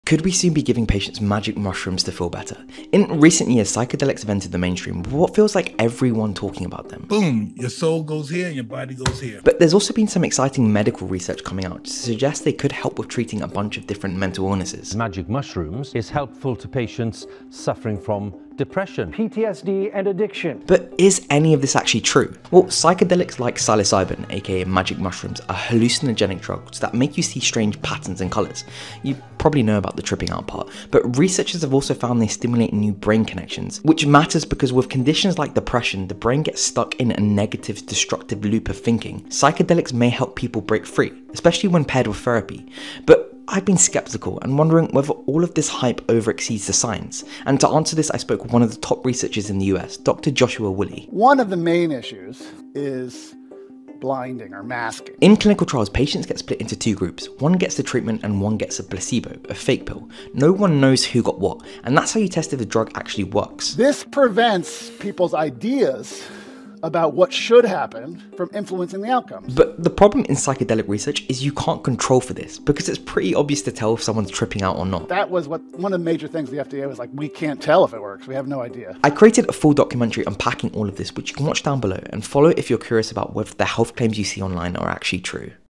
Can We Cure Depression With Psychedelics?
In recent years, psychedelics have exploded into the mainstream. You’ve probably seen the headlines — “Psilocybin can rewire the brain,” “Magic mushrooms could cure depression,” — but is there real science behind the hype?

In this short, I break down the surprising medical side of magic mushrooms — also known as psilocybin — and why researchers are investigating them as a potential treatment for depression, PTSD, and addiction.

It’s true that psychedelics can make you hallucinate. But some studies suggest they also stimulate new neural connections — potentially helping people escape from destructive loops of thinking. That’s why researchers are pairing them with therapy in clinical trials.

But here’s the catch: how do you properly test a psychedelic drug when it’s obvious who’s taken it and who hasn’t? I spoke to leading U.S. researcher Dr. Joshua Woolley, who explains why this “blinding problem” is one of the biggest challenges in psychedelic science — and why the FDA still isn’t convinced.

This short is part of a bigger documentary I made investigating whether psychedelic therapy is the future of mental health — or just another overhyped trend.
📩 Follow for more evidence-based health explainers that cut through the noise.